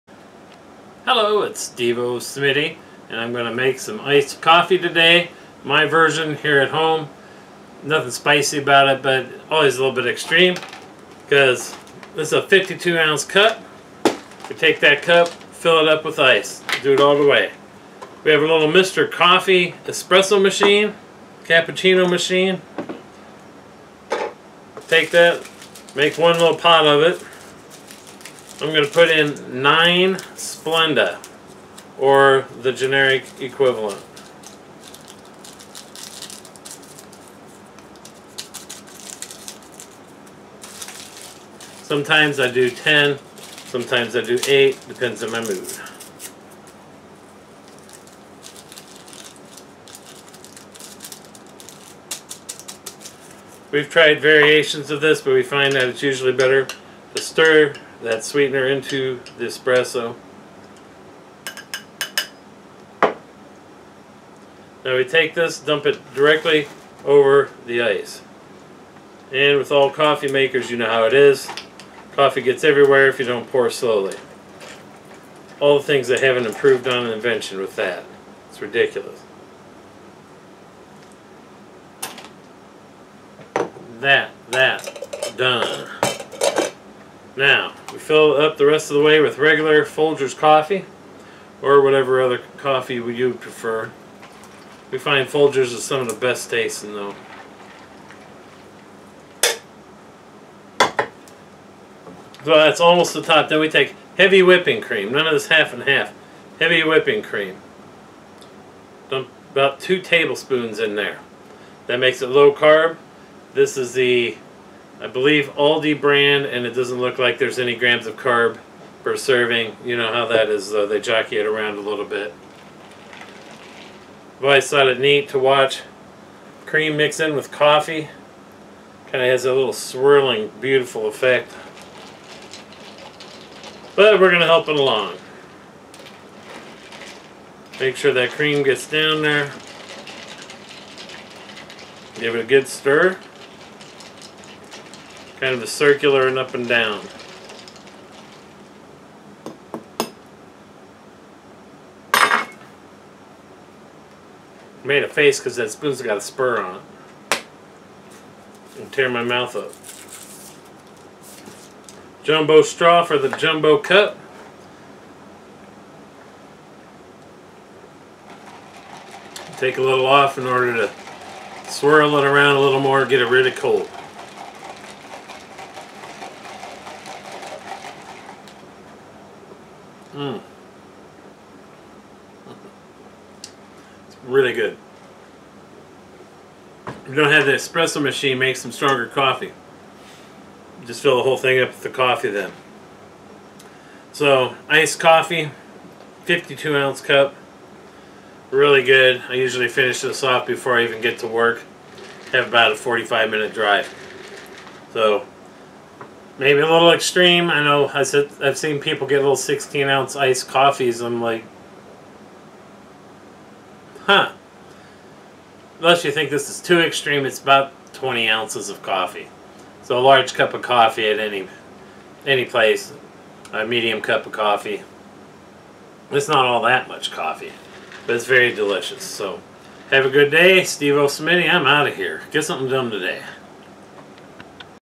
Recipe: XL Low Carb Iced Coffee
Going low carb with this one, just a simple XXL Iced Coffee. Forget the tiny 16 oz. iced coffees. 52 oz. Go big or go home.
Ingredients:
Ice to fill 52 oz cup
8 oz esspresso
9 splenda
coffee to almost the top
2 Tbs. heavy cream

Zero carbs. Lots of happiness

(alt spelling: steveosmitty)